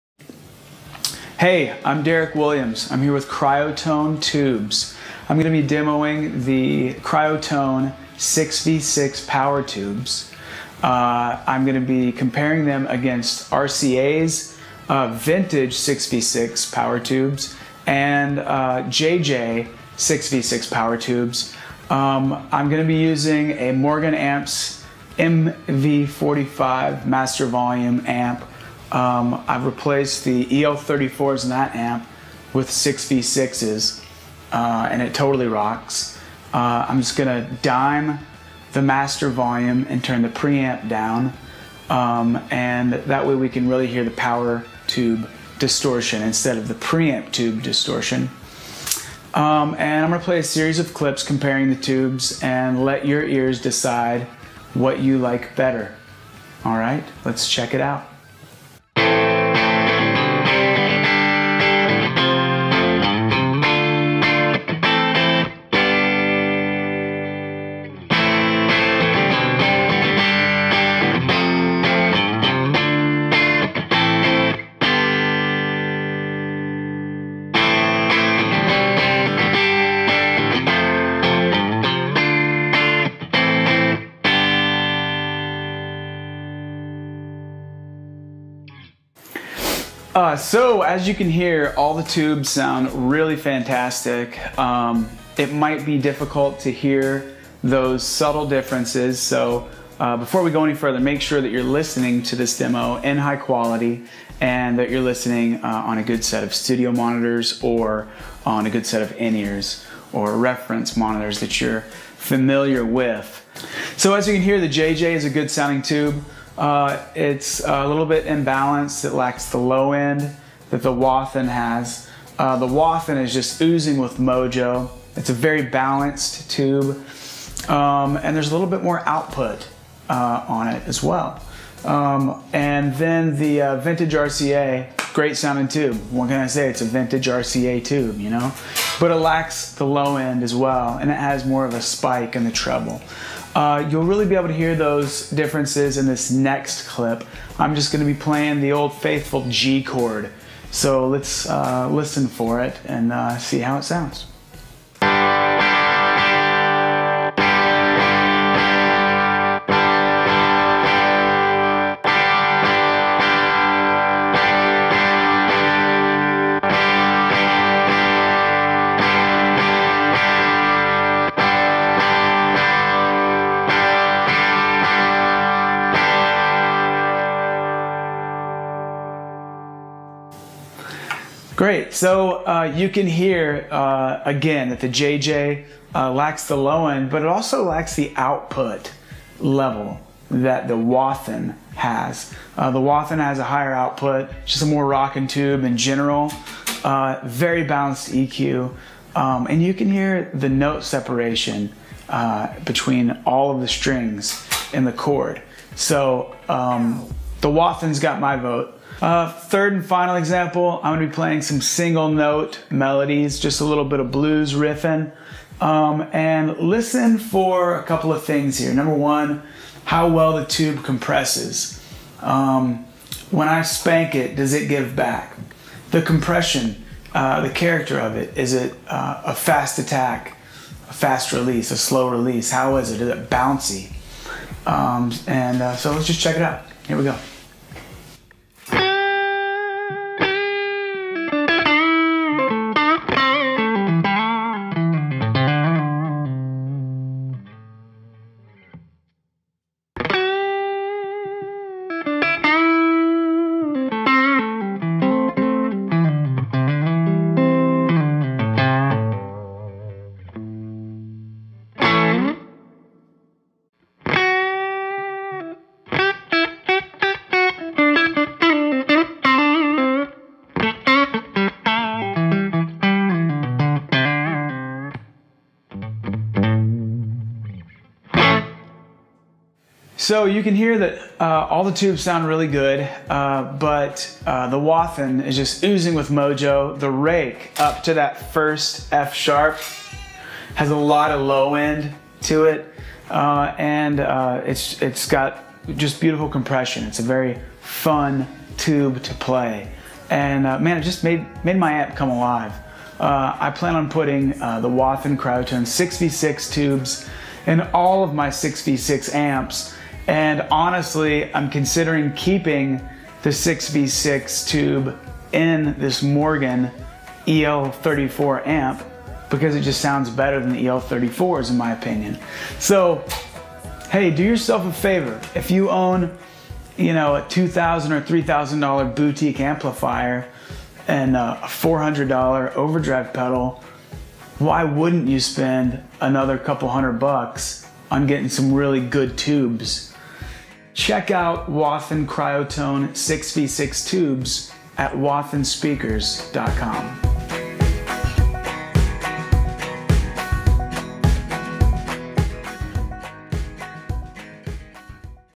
Wathen Cryotone 6V6WC Demo by Derek Williams
The Wathen CryoTone brand 6V6WC is just a step above the rest.

Thanks to Derek Williams for taking the time to knock out a great comparison of what we bring to the table.

Get yours ordered today!